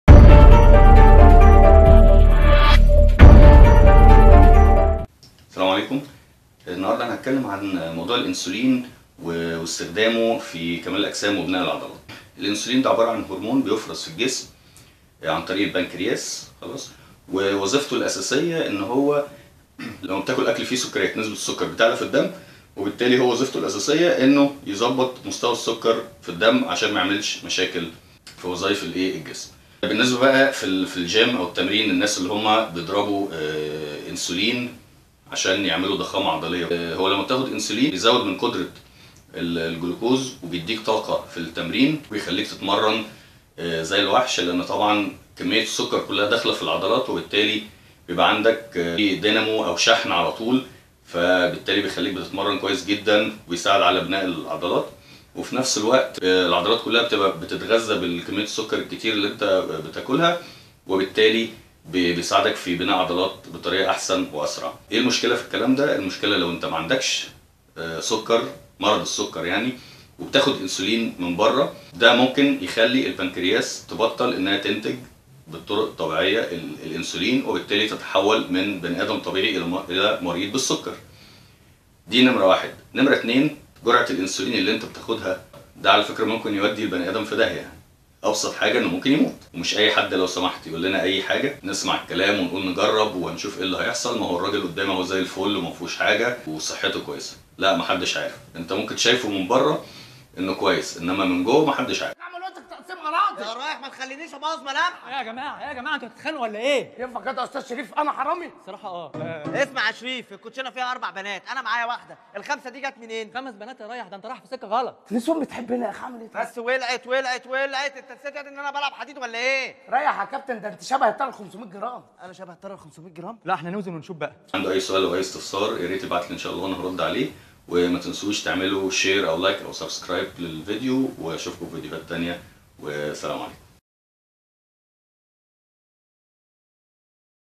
استخدام الانسولين في تضخيم العضلات و كمال الاجسام
الانسولين insulin هو هرمون يفرز من البنكرياس ليساعد على تنظيم مستوى السكر فى الجسم و يستخدم فى علاج مرض السكر ويستخدم بكثرة بين لاعبى كمال الأجسام لما له من تأثير بنائى للعضلات فهو يساعد الجسم علي امتصاص الاحماض الامينية و الجلوكوز.

Mohamed Shaalan
Certified Personal Trainer & Fitness Nutrition Specialist